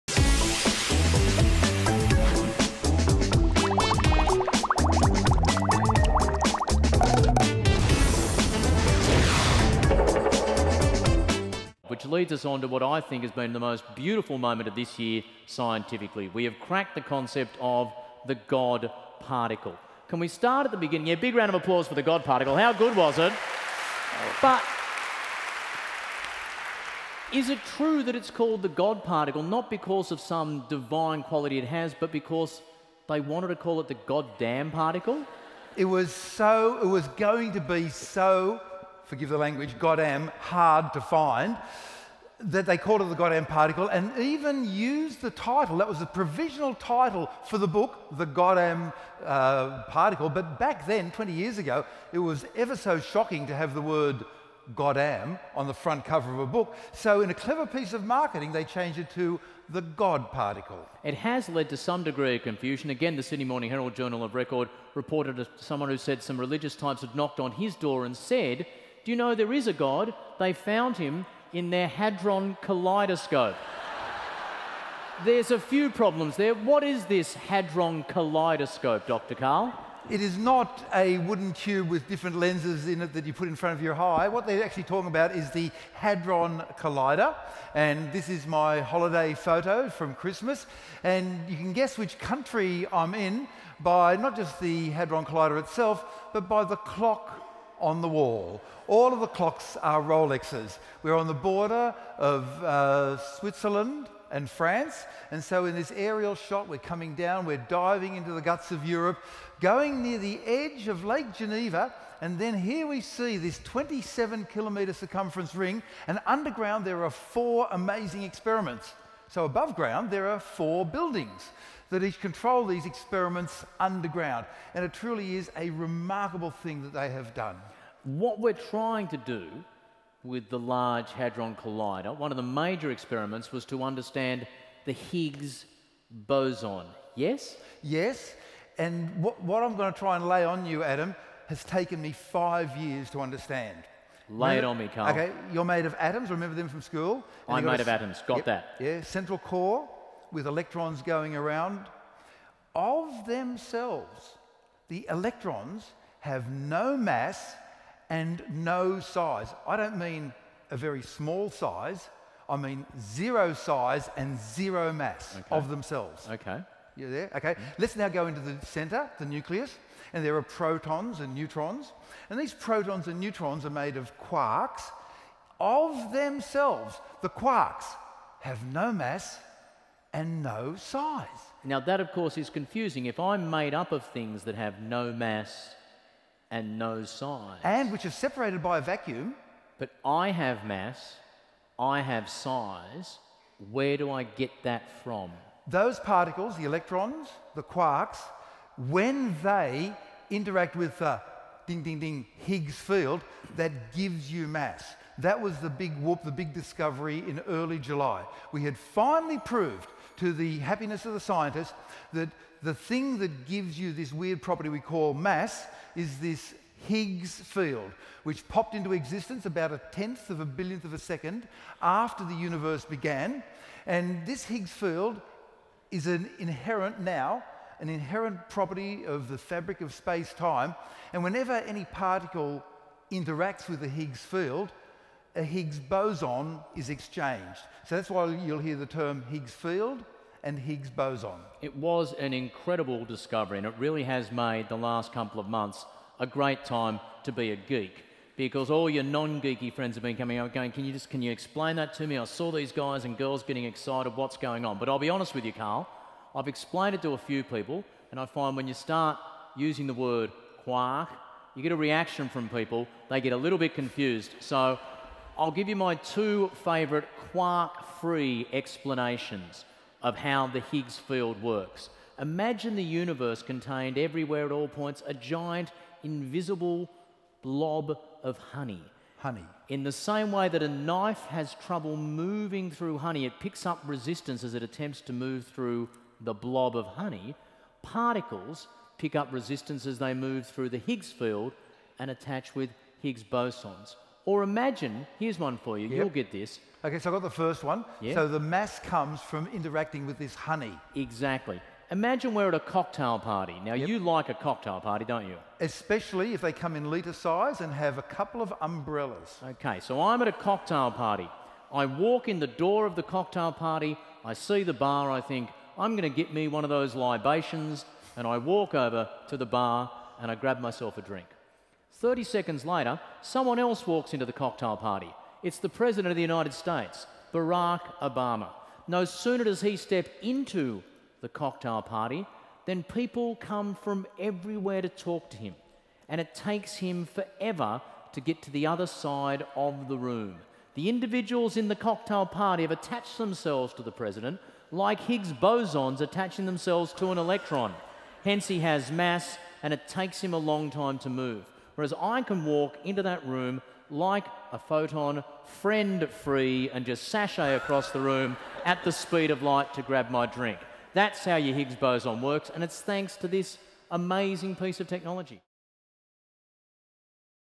The God Particle & Higgs Boson: Part 5 of The Sleek Geeks celebrate National Science Week
In a massive night of science and comedy, Sleek Geeks Dr Karl and Adam Spencer celebrated National Science Week with a sold out show at Sydney Town Hall. In this episode, they demystify the God Particle and Higgs Boson.

This event was hosted by the University of Sydney and The City of Sydney.